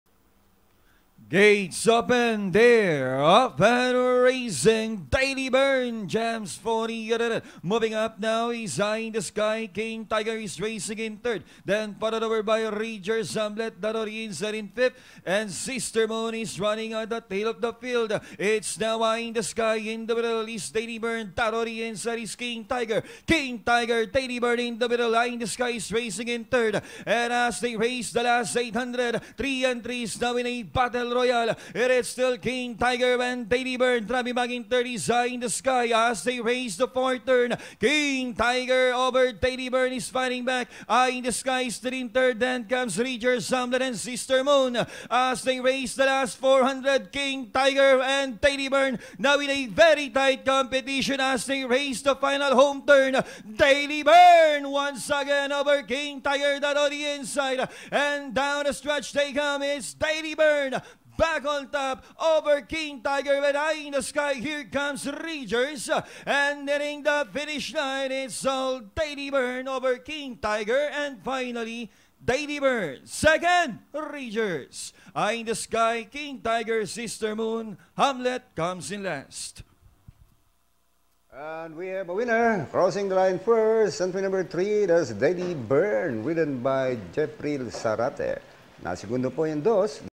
2024 May 05 | R01 - DAILY BURN (3) [1:11.8]
MMTCI - Race #01 - PHILRACOM-RBHS CLASS 4 (34-39)
Winning Horse - DAILY BURN (3)
Jockey : J T ZARATE
Finish: 3/2/5/4